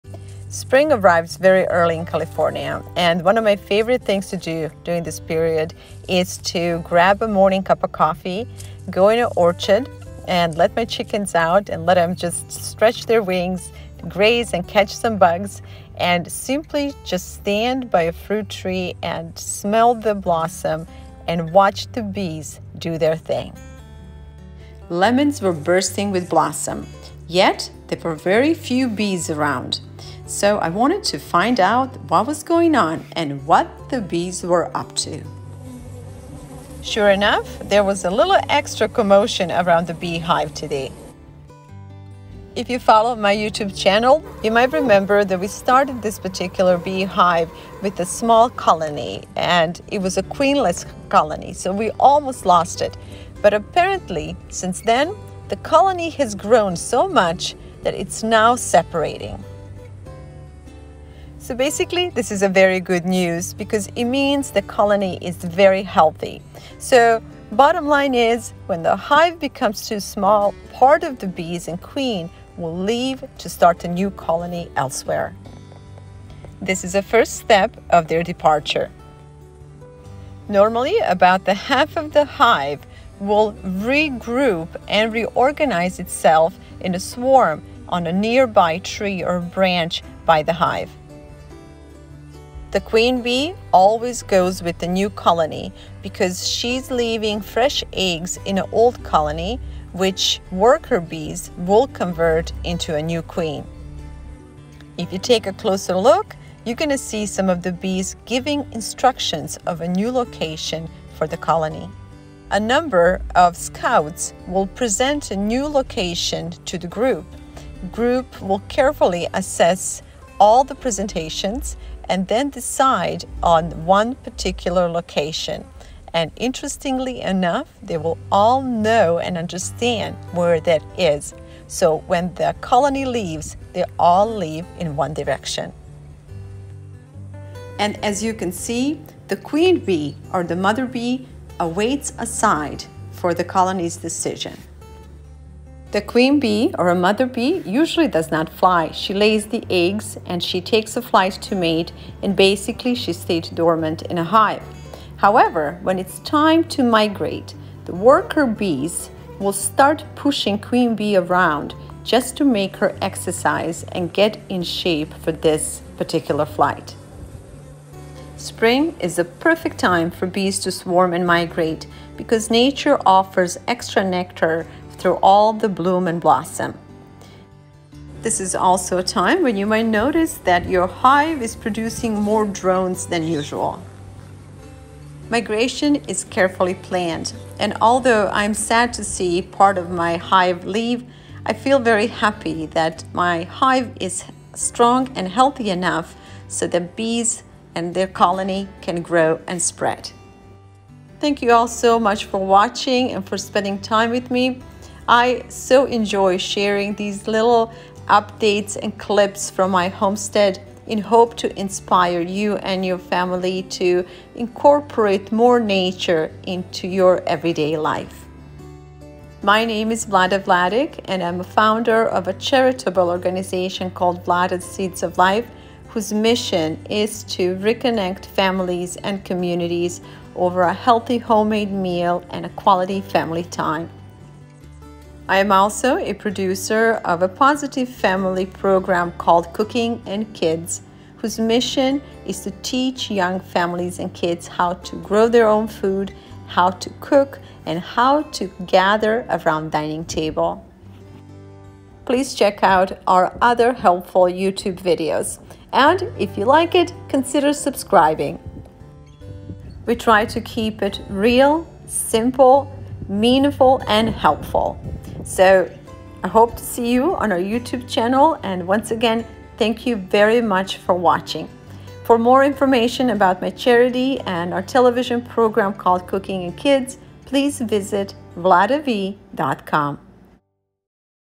WHY BEES SWARM - simple explanation
Easy to understand bee world and how it works.